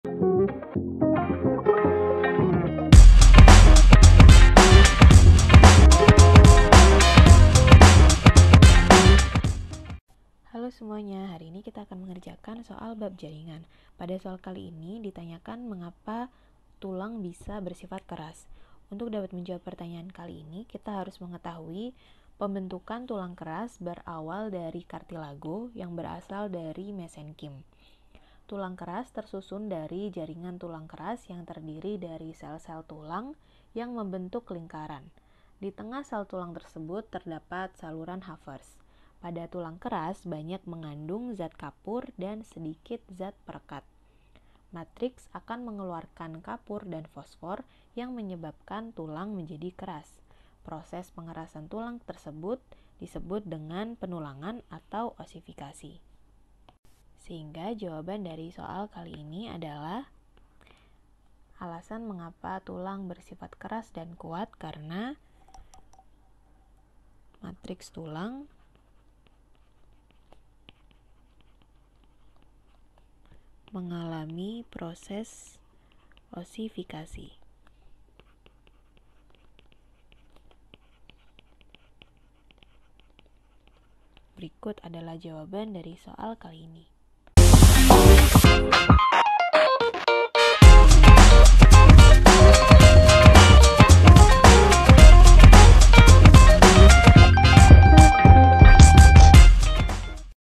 Tulang bersifat keras dan kuat karena ... ... | Pendalaman Buku | Kelas 11 | 2 | Biologi
✅ Dalam video ini kita akan membahas: Tulang bersifat keras dan kuat karena ...
a. matriks tulang mengalami klasifikasi ;b. matriks tulang mengalami kapur dan serabut kolagen ;c. adanya lamela dan kanalikuli ;d. sel tulang memiliki dinding sel yang tebal ;e. matriks tulang tersusun sangat rapat
Tema:
Kelas: 11
Mata Pelajaran: Biologi
Bab: 2
Buku: Pendalaman Buku Teks Biologi 2A SMA Kelas XI

👉 Buku yang telah tersedia:
{PLAYLIST_LINK}
Di mana kamu dapat menemukan kami?